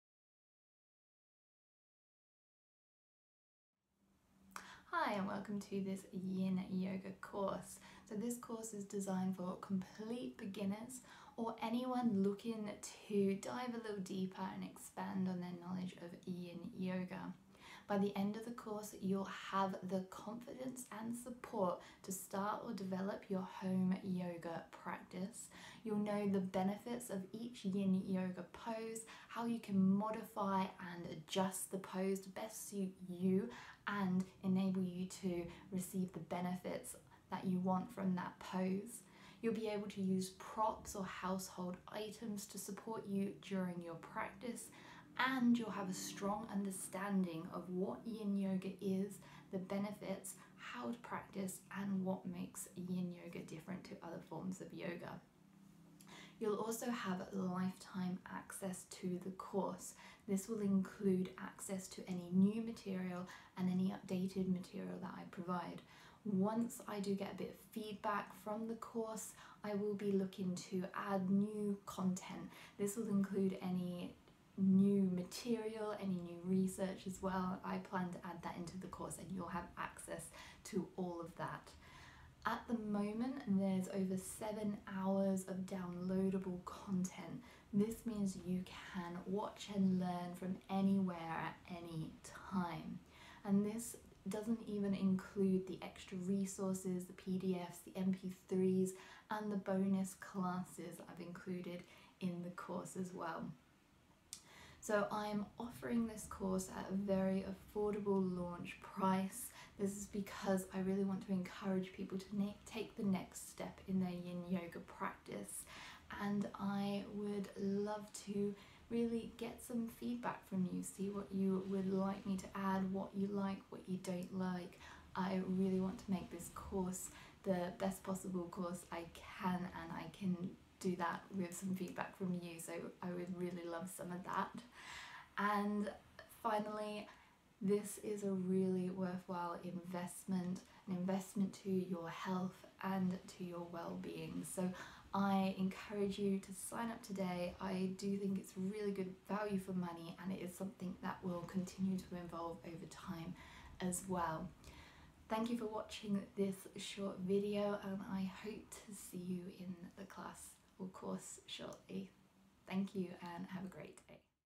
Yoga of the Joints: Yin Yoga Course for Beginners
This course is designed to complete beginners either new to yoga or Yin yoga, or those students wanting to dive a little deeper into Yin yoga. The course will give you the confidence and knowledge to start or expand on your Yin yoga practice and help you to develop your home Yin yoga practice.

Why practice Yin yoga?

* Yin yoga calms the nervous system by engaging the parasympathetic nervous system. This switches off our 'fight or flight' response, reducing feelings of anxiousness, stress and overwhelm.

* Yin yoga can quieten a busy mind - the practice is a meditative practice and results in a lot of the same benefits as meditation. It is also a great way to build on or start a meditation practice.

* Yin yoga targets the connective tissues, such as the fascia, which supports the joints. This can help with joint health, mobility and range of motion - the reason Yin yoga often is referred to as 'Yoga of the Joints'.

By the end of the course you will:

- Know how to modify and adjust poses to best benefit you (this includes how to use props and household items to support you).

- Be confident in your home practice and be able to create your own Yin yoga sequences, if you like.

- Have an understanding of Yin yoga, what it is and how it can benefit you.

You'll receive:

- x3 lessons on what Yin yoga is, the benefits, how to practice and how to prepare for the course.

- A lesson on the meridians and how they relate to/ are used in Yin Yoga (plus example classes demonstrating how the meridians are used in Yin yoga).

- x25 In-depth tutorials covering over 35 Yin yoga poses. Each tutorial will cover where the pose targets, how you can modify the pose and some alternative poses you can take.

- x3 lessons on how to use breath work in your Yin yoga practice and some of the benefits of doing so.

- Information and tips on meditation and how to incorporate this into your practice.

- A downloadable guided relaxation (Yoga Nidra).

- x3 self-led Yin yoga classes to get you started with your home practice.

- x3 full length Yin yoga classes.

- Over 7 hours of downloadable content (over 40 lectures), so you can learn and practice anywhere, at anytime.

- Additional resources and PDFs

- Lifetime access to the course including to any new or updated content.

- You'll also receive two BONUS Yin yoga classes (in addition to those included in the course)

This is quite an in-depth course, which I plan to be updating based on feedback and suggestions. As you'll have lifetime access, you'll have access to all these updates and new content!

I'm offering an affordable launch price, which will give you lifetime access to the course, including access to any new or updated content. Making this offer a worthwhile investment into your health and wellbeing.